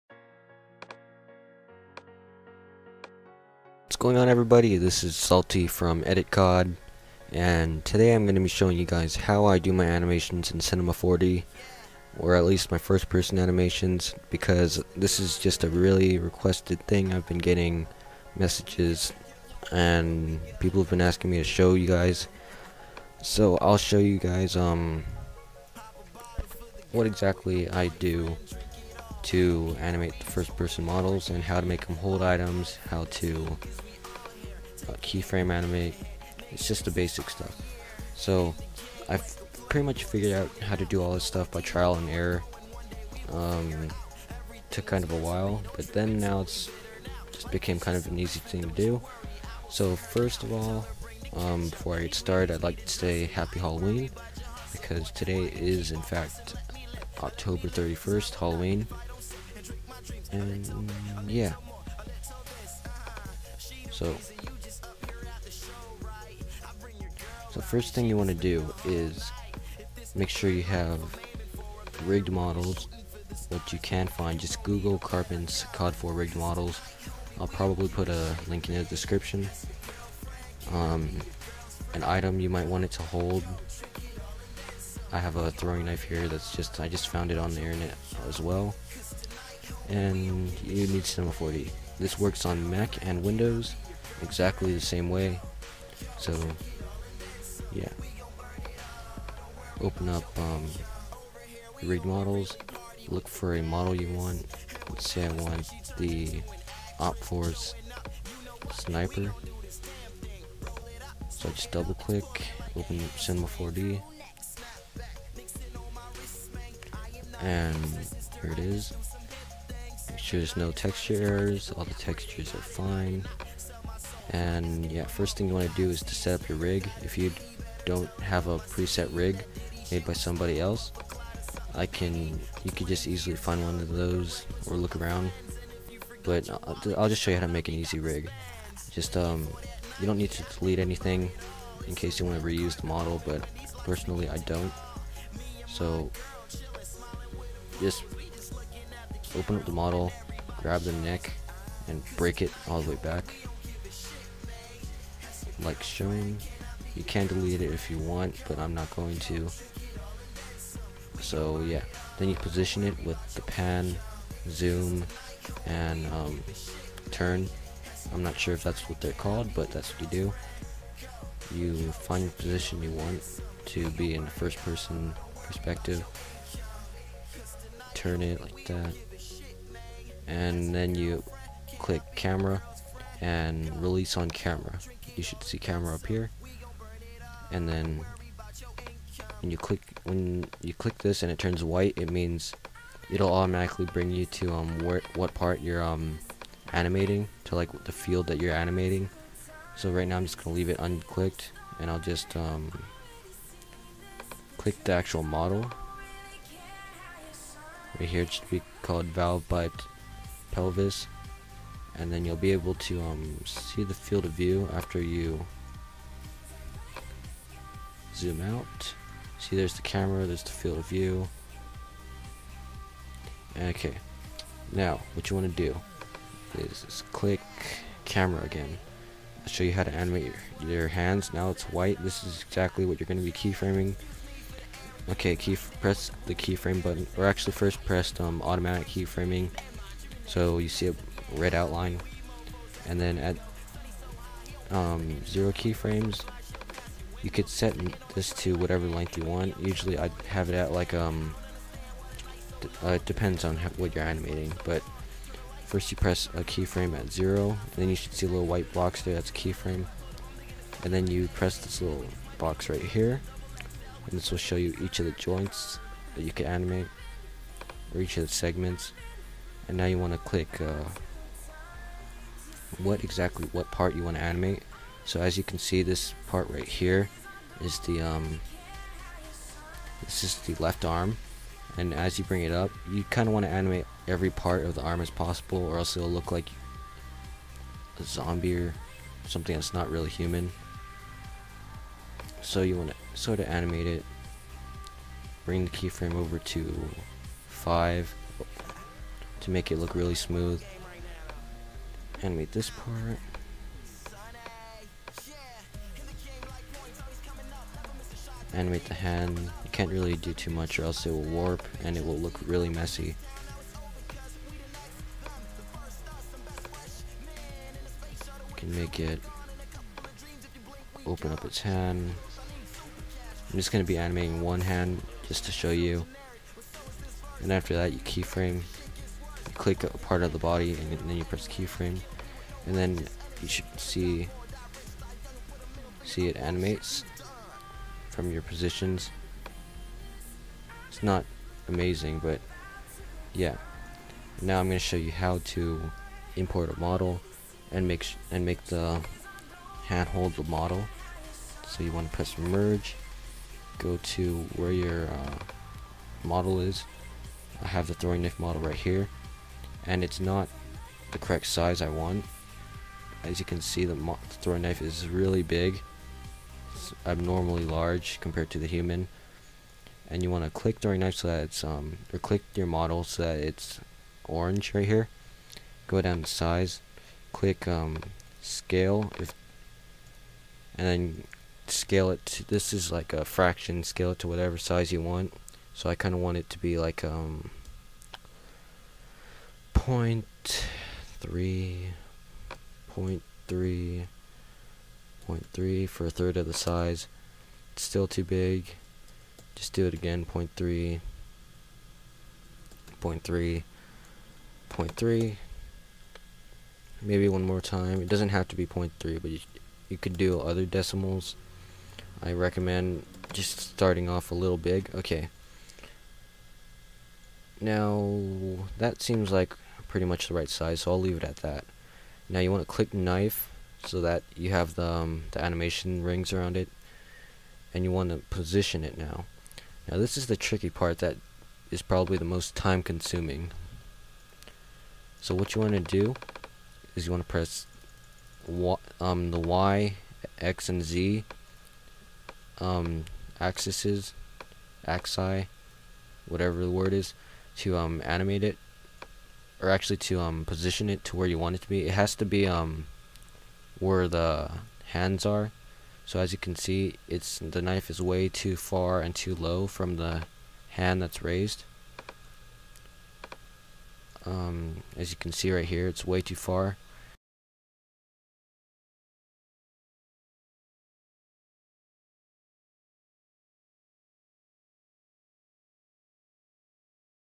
C4D Tuts- How to animate a FP Model(PART 1-Salty)
this is part one to a series of videos about how i do my magic.
The Models are in my animation pack.